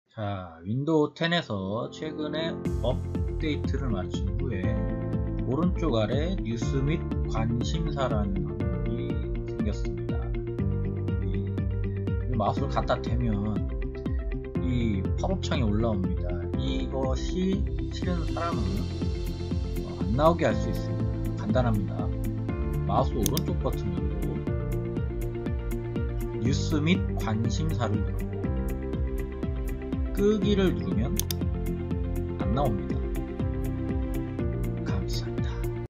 뉴스 및 관심사 안나오게 하는 방법
윈도우 업데이트 후 작업표시줄 우측 하단에 새로 생긴 뉴스 및 관심사 기능을 끄는 방법을 설명드립니다.